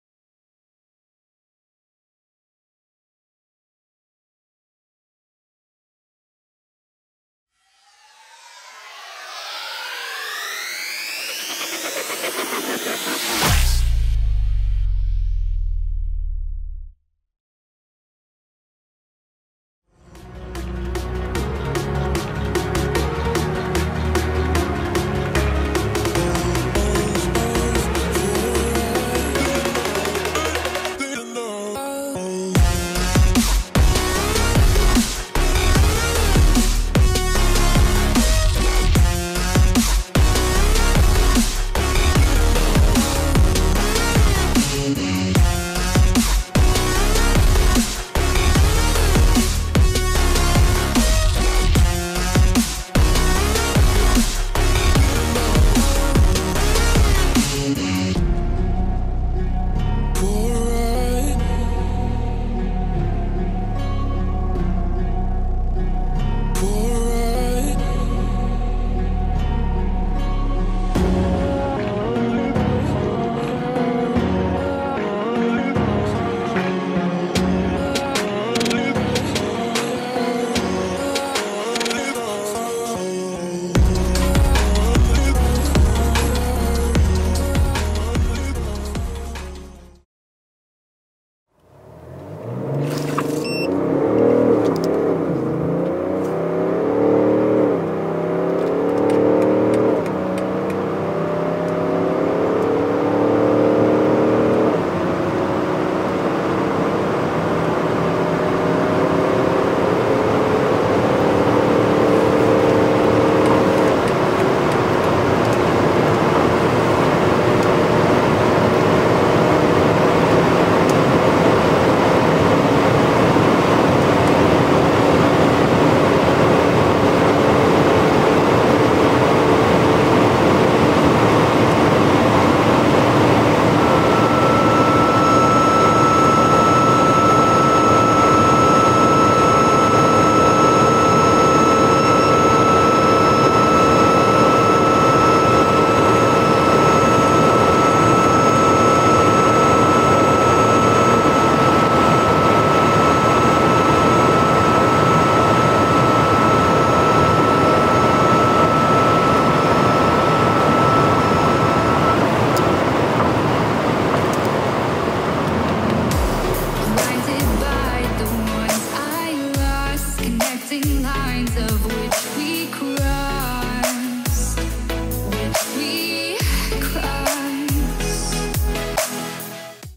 0-250 km/h | BMW X5 M50D xDrive | TOP SPEED and Acceleration TEST✔
2019 BMW X5 M50D xDrive (3.0 Quad T Diesel, 294kW, 400Hp, 760Nm) 0-250 km/h on German unrestricted Autobahn (Highway).

Time measure with: QSTARZ LT-6000S and BT-Q818XT.